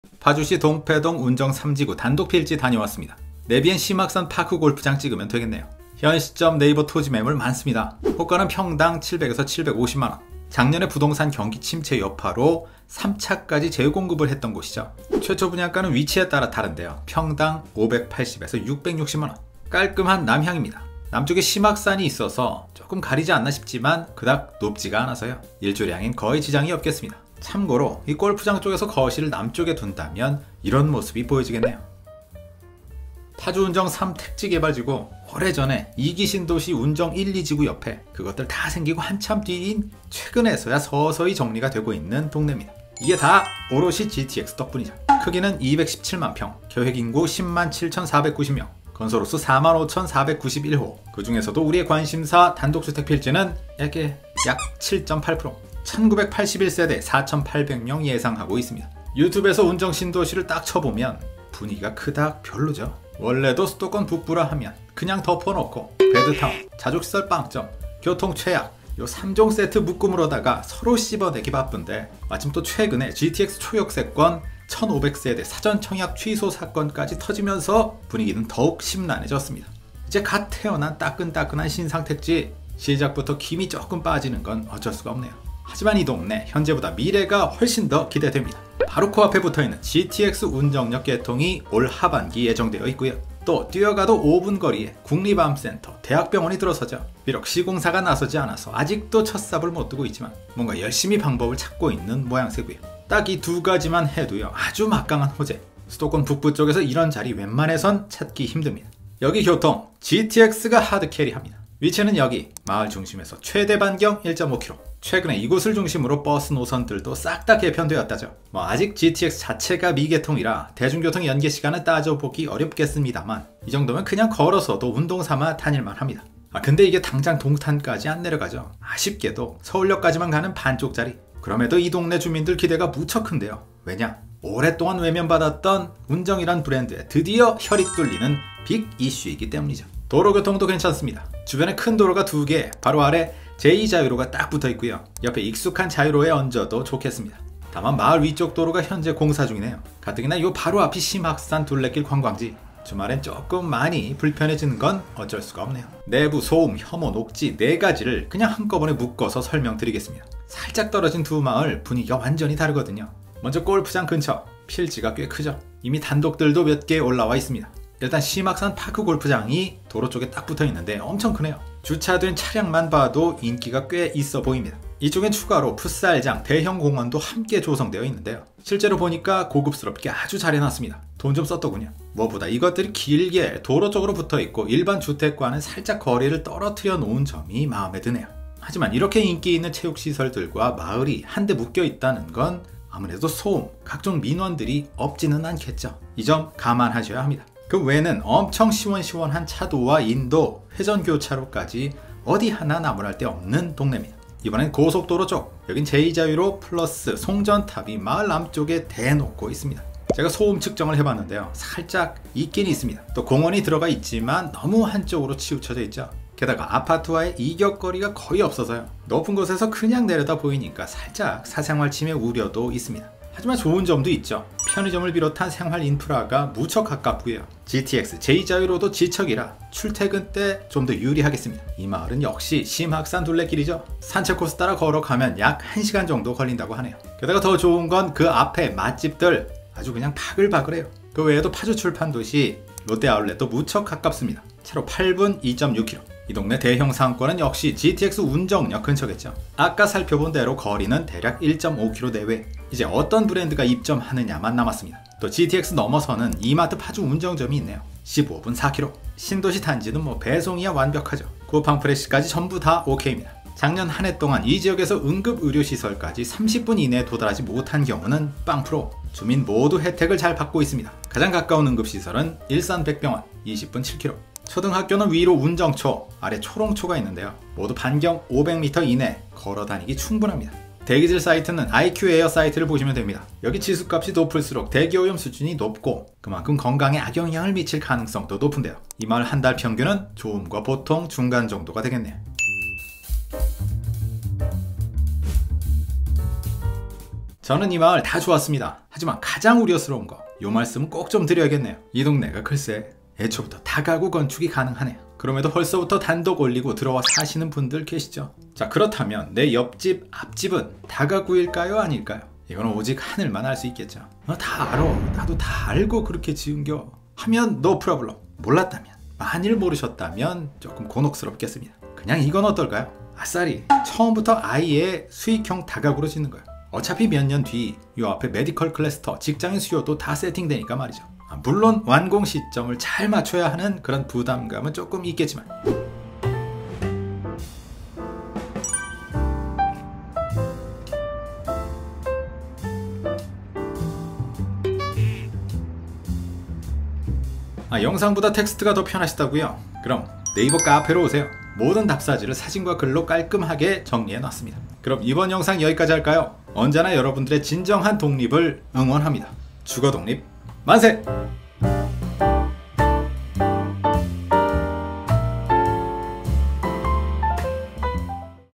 '와.. 나만 몰랐네?' 이동네는 '이것' 모르시면 큰일납니다 (마을답사 157)
★팩트에 혼선이 우려되어 정리합니다.
-심학산파크골프장 마을(C6, C7, C8구역): 2층 3가구 이하 가능 지역
-제2 자유로 인근 마을(C5구역): 3층 5가구 이하 가능 지역